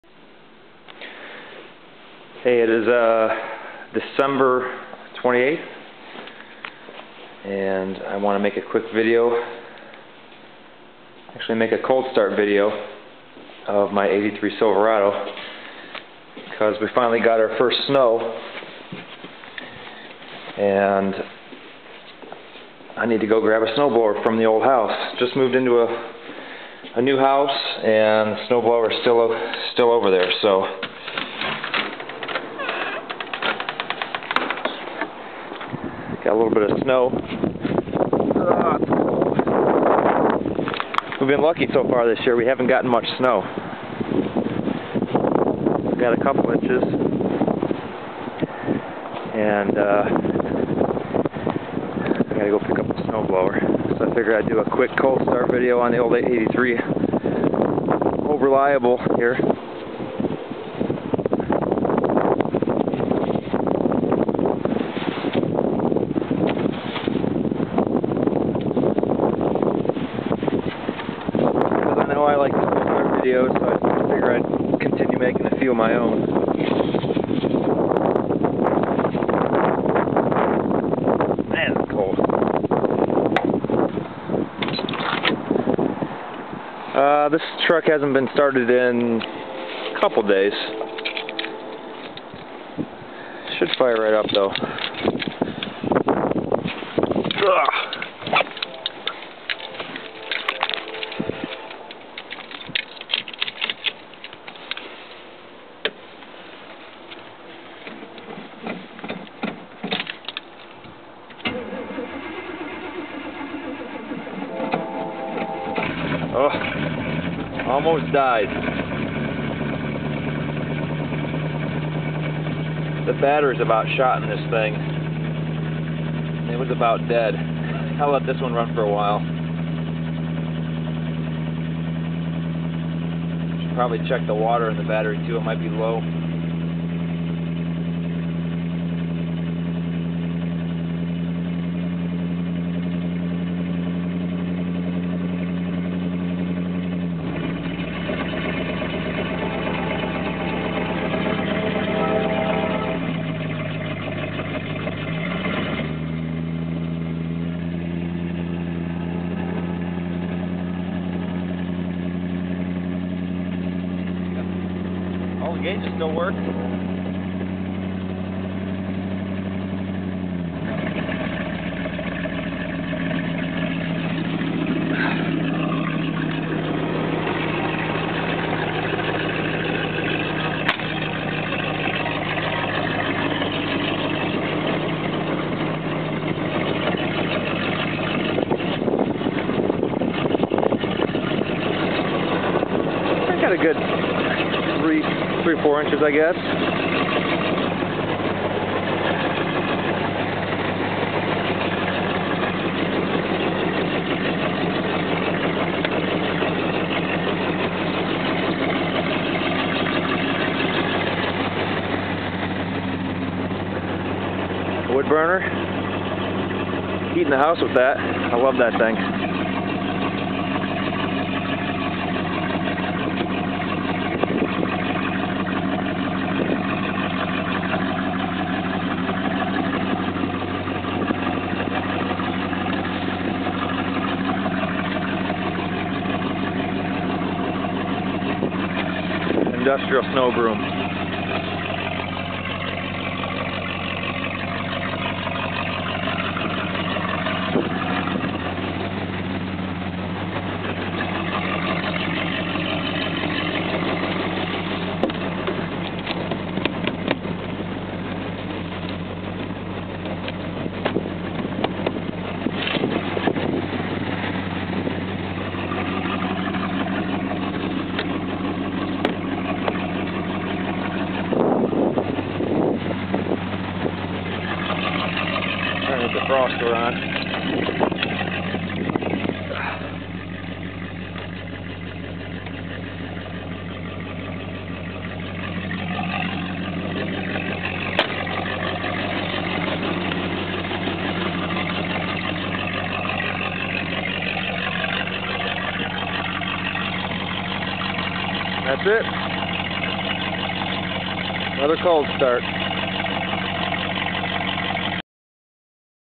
Cold Start 1983 Chevy Silverado Classic G-Body Garage
Yet another cold start in my 83 Silverado. Enjoy!